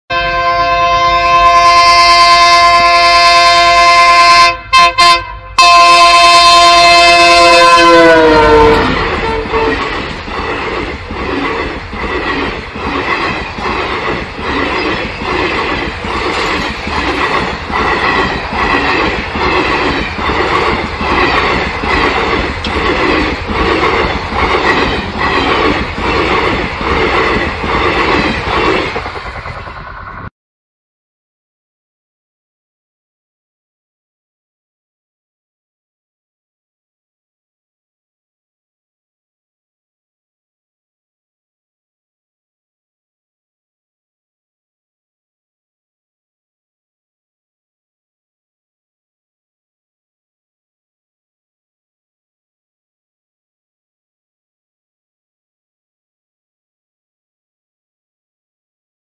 NEPALI TRAIN
Nepali train is arriving on on the Janakpur station today..
Stay updated Nepalese, it is in Jayanagar now, as per the source...